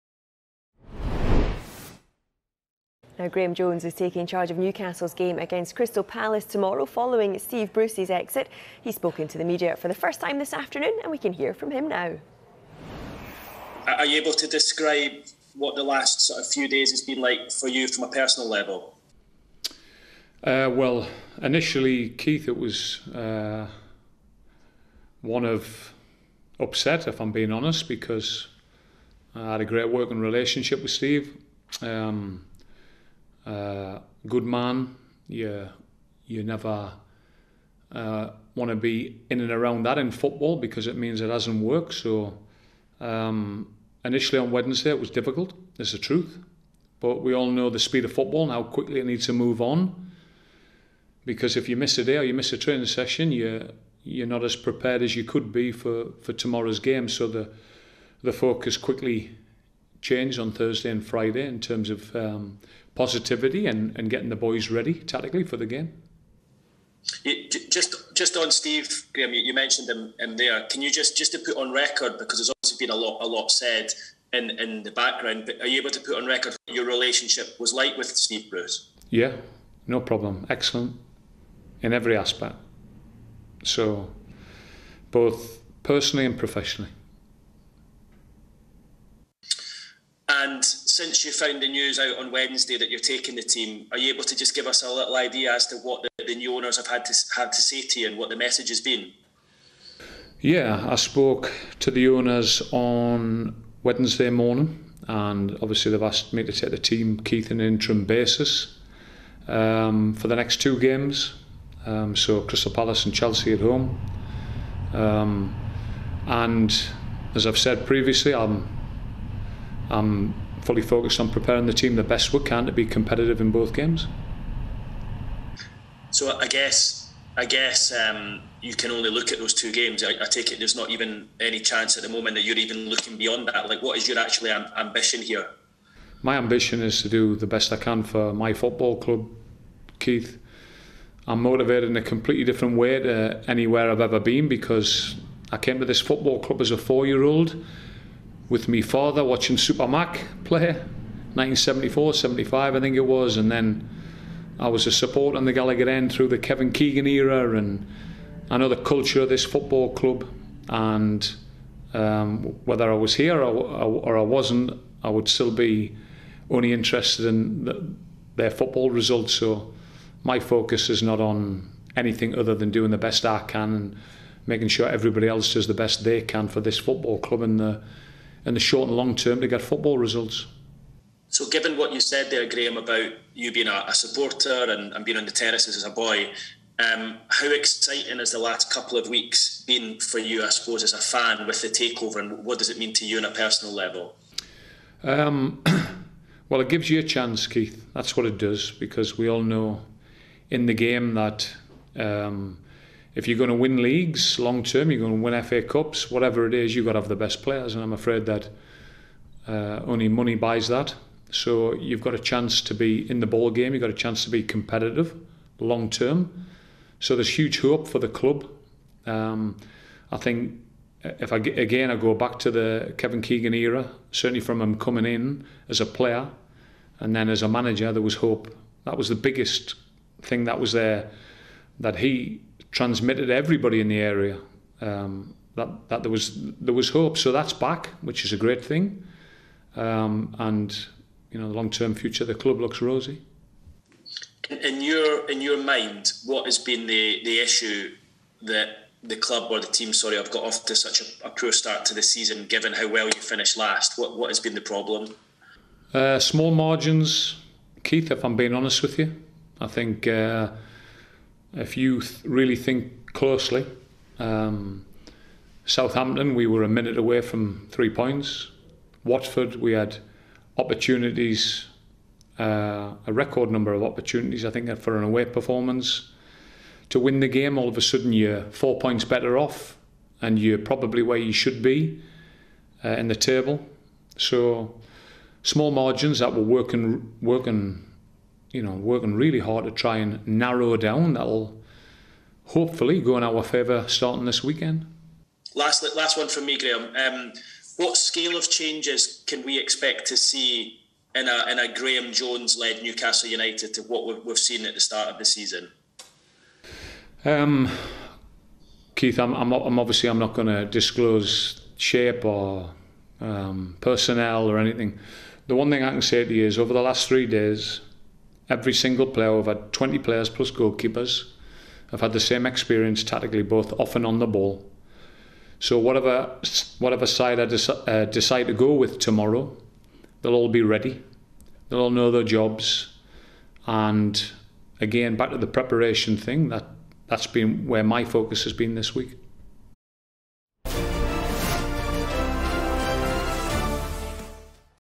Graeme Jones to take charge of Newcastle's next 2 games
Speaking to the media for the first time since being appointed as Newcastle's caretaker manager, Graeme Jones has confirmed that he has been asked to lead the club in their games against Crystal Palace & Chelsea.